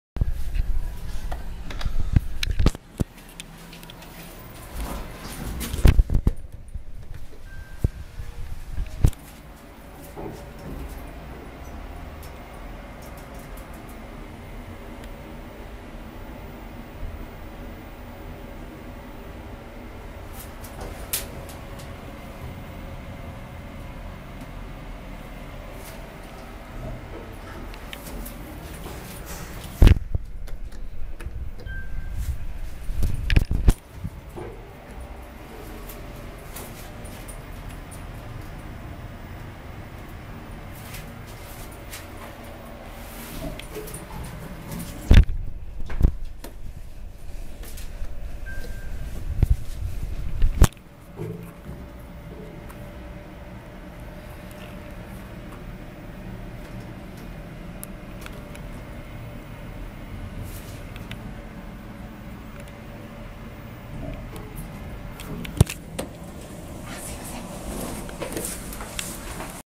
福山市内某ビルのエレベーターvol.24
車椅子非対応ですが、標準型の鏡があります。

メーカー：三菱
機種：エレパックアイ
駆動方式：ロープ式
設置台数：1基
アナウンス：なし
到着チャイム：なし
車椅子操作盤：なし
停止階：1,2,3,4
停止可能階：1,2,3,4
用途：乗用
定員：9名
積載：600kg
ドア：2枚片開きドア
設置年度：2002年12月